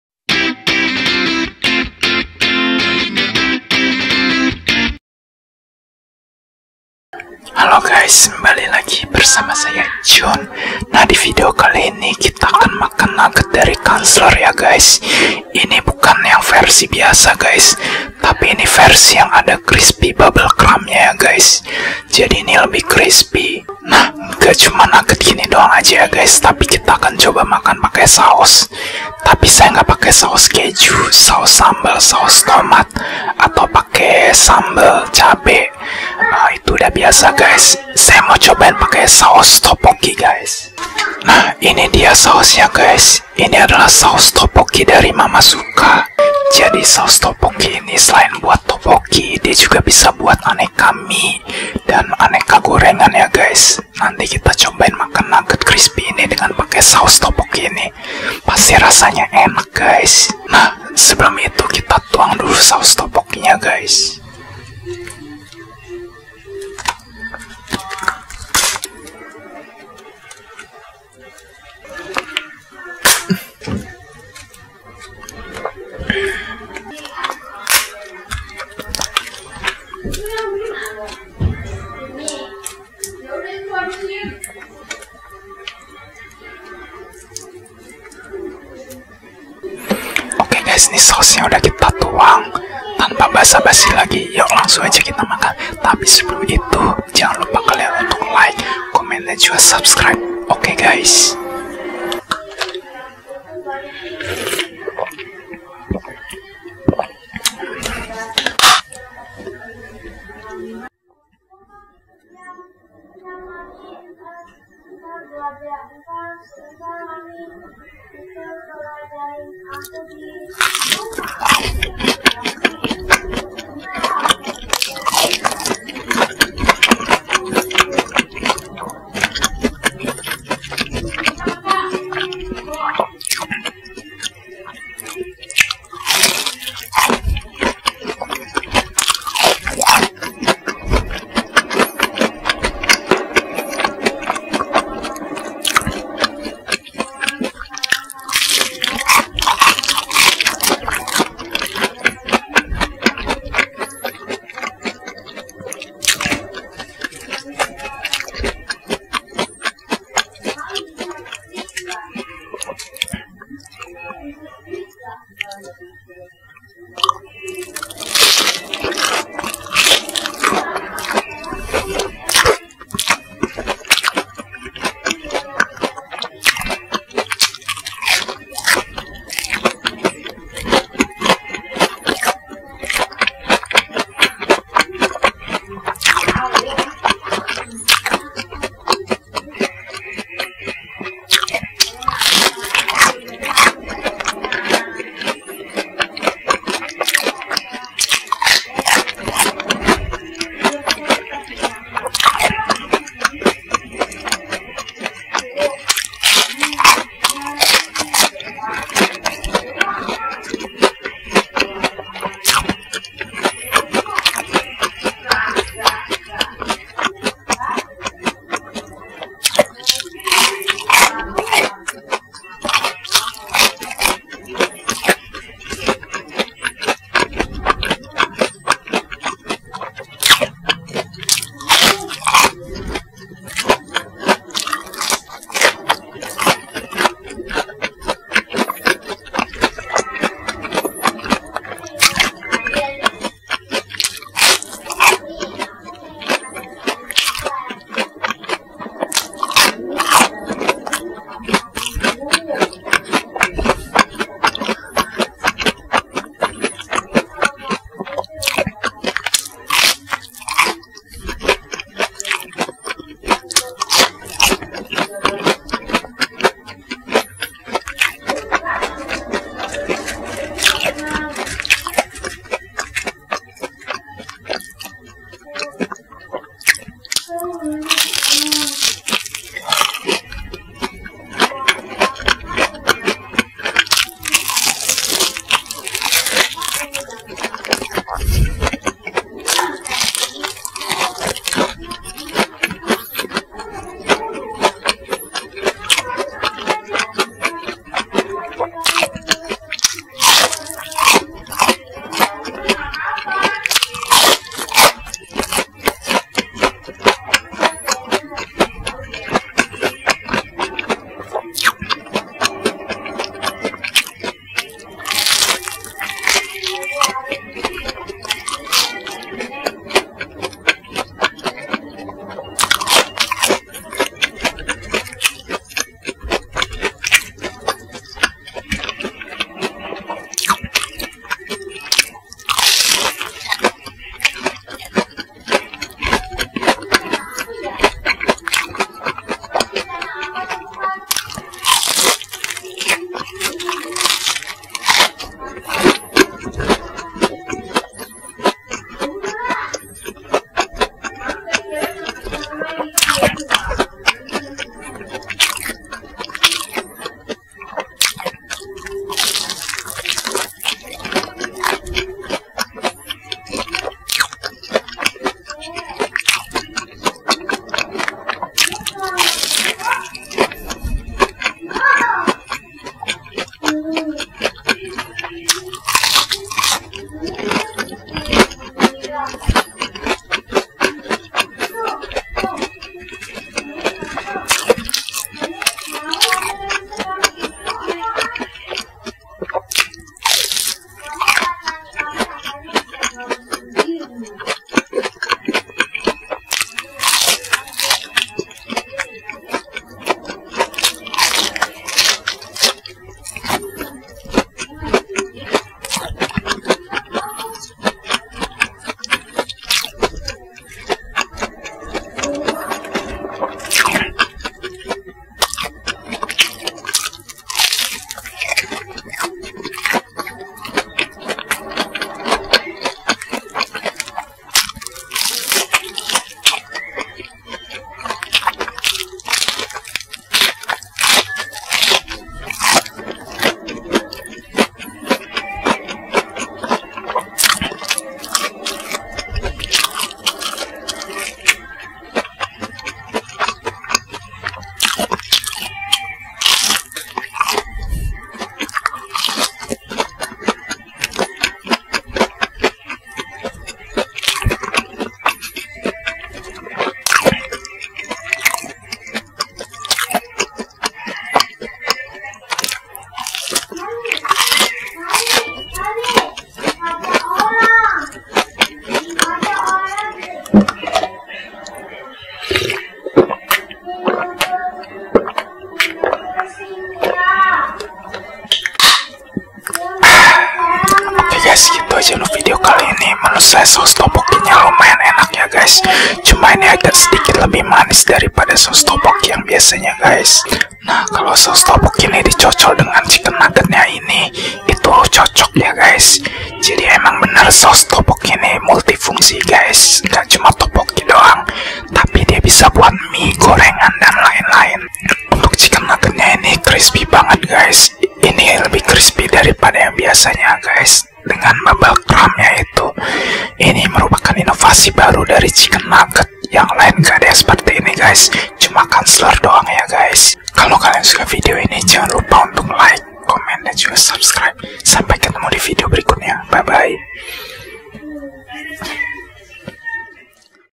SUPER CRISPY! ASMR CHICKEN NUGGET KANZLER DENGAN SAUS TOPOKKI | ASMR INDONESIA
Halo Gaes,
Di video kali ini saya mau makan chicken nugget dari Kanzler. Yang ini adalah varian yang crispy, jadi ada balutan crispy bubble crumb diluarnya.

Makannya dicocol pake saus topokki dari mamasuka, rasanya seperti apa?

Selamat menonton!